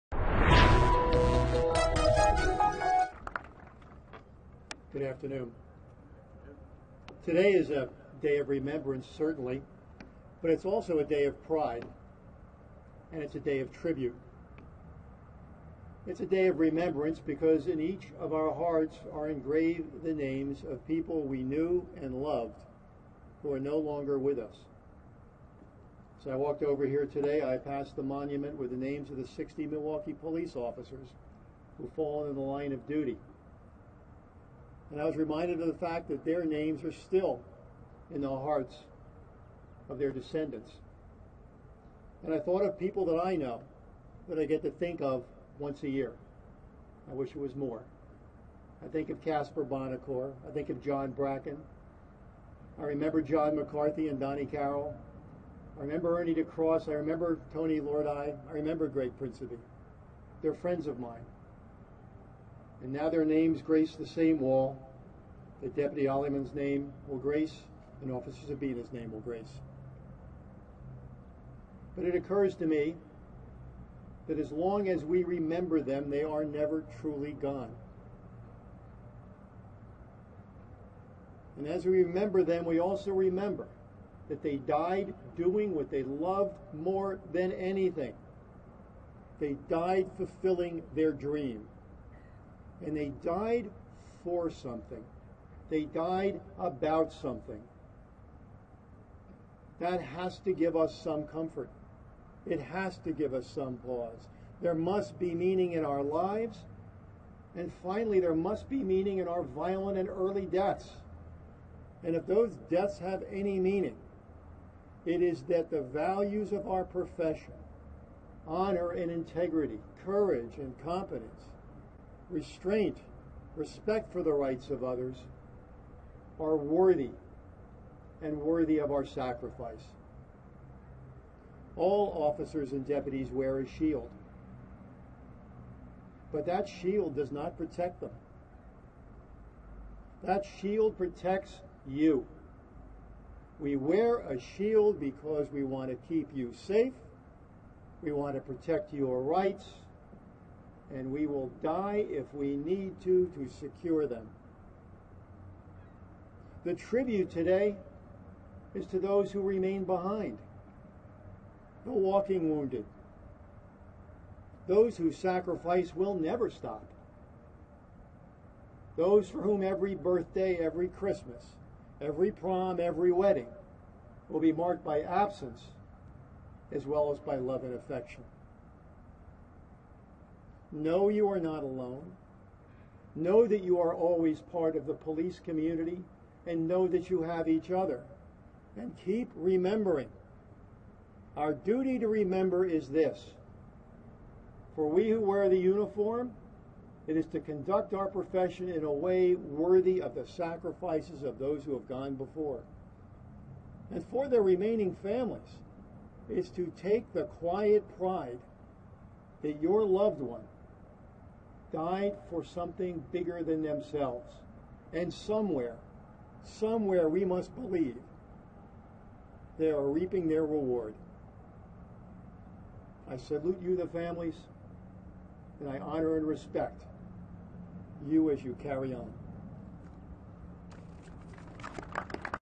RAW: Chief Flynn speech at officers ceremony
RAW: Milwaukee police chief Ed Flynn's speech at officers ceremony